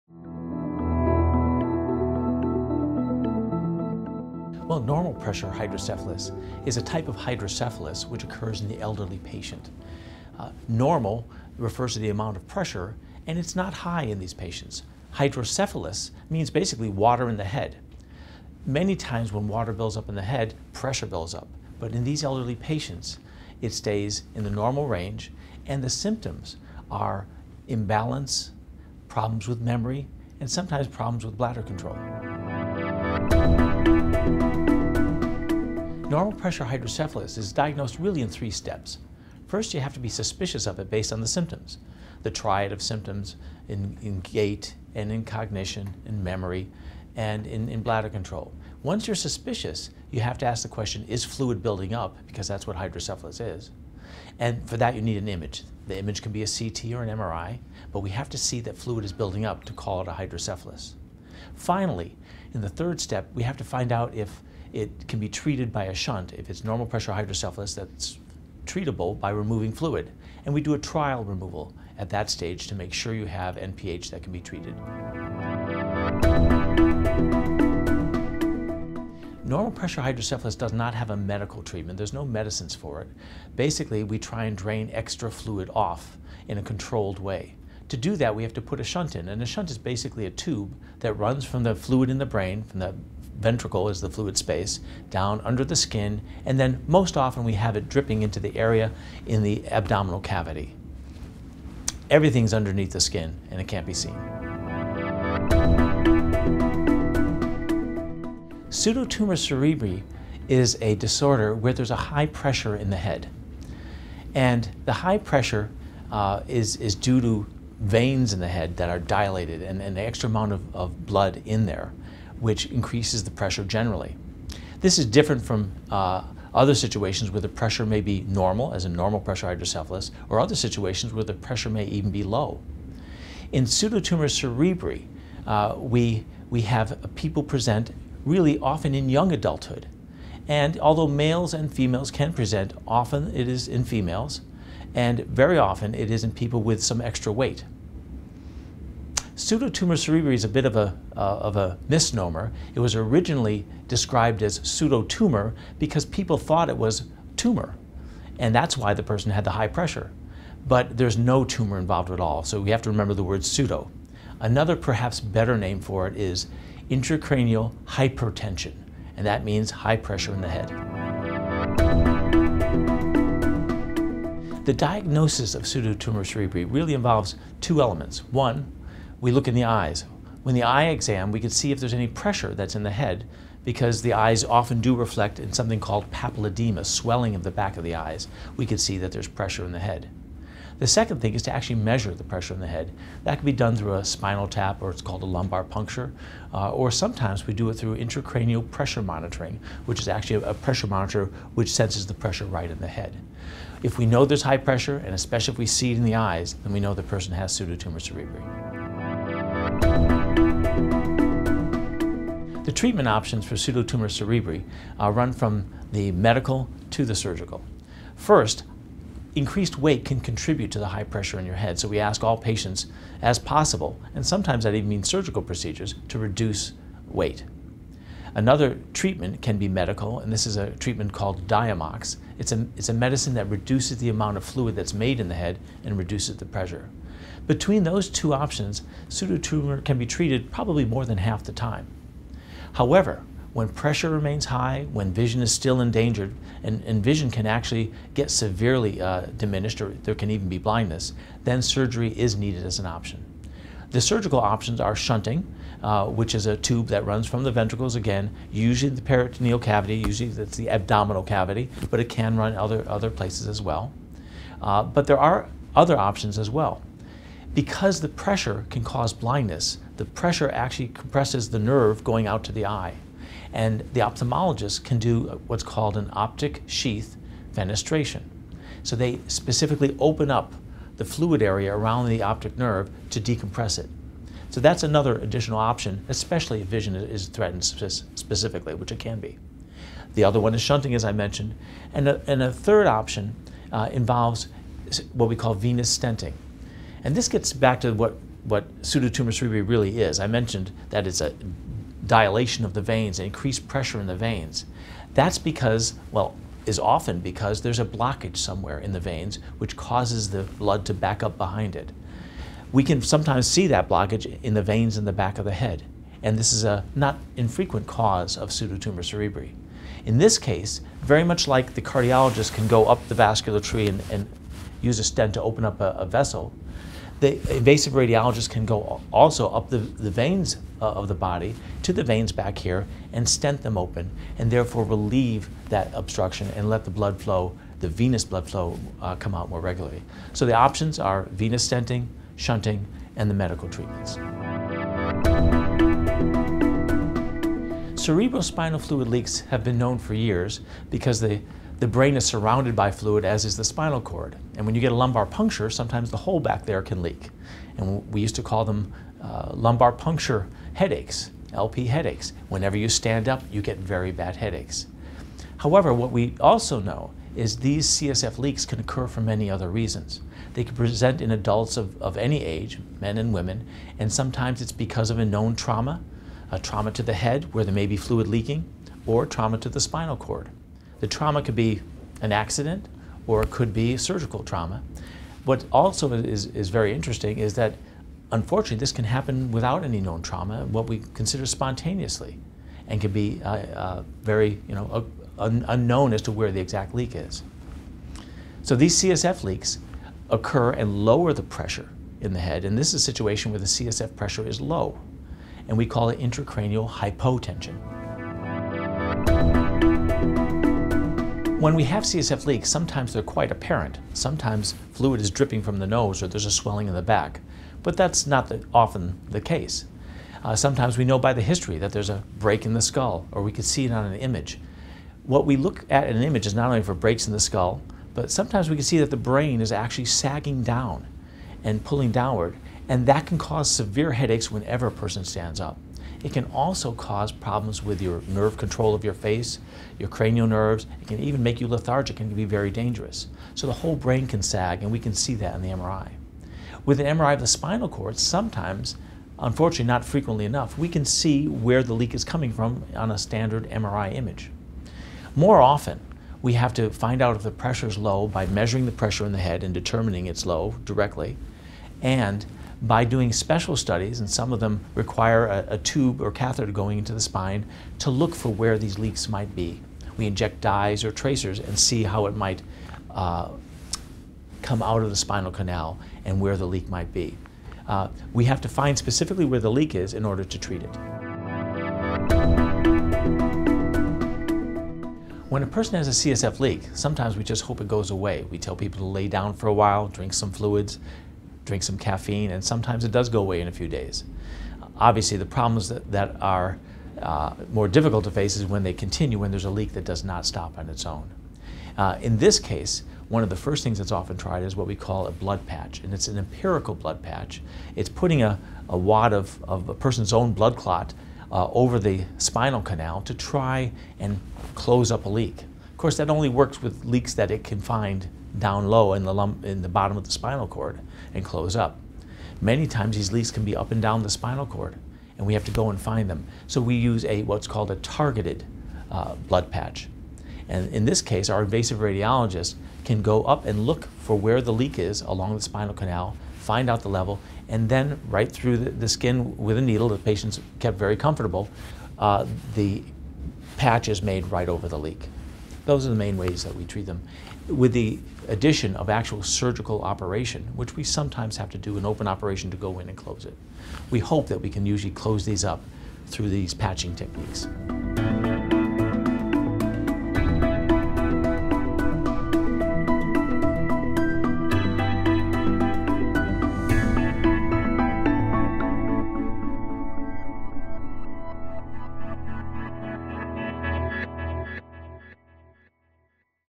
Cerebral Spinal Fluid Disorders: Frequently Asked Questions
Johns Hopkins neurosurgeon Mark Luciano answers commonly asked questions about normal pressure hydrocephalus, pseudotumor cerebri and cerebral spinal fluid leaks, including how these disorders are diagnosed and treated.

Questions Answered:
1. What is Normal Pressure Hydrocephalus? 0:03
2.  How is Normal Pressure Hydrocephalus Diagnosed? 0:33
3. What is Pseudotumor Cerebri? 1:46
4. What are the treatment options for Pseudotumor Cerebri? 3:31
5. What are Cerebral Spinal Fluid leaks? 5:46
6. How are Cerebral Spinal Fluid leaks diagnosed? 6:51
7. What are the treatment options for Cerebral Spinal Fluid leaks? 8:04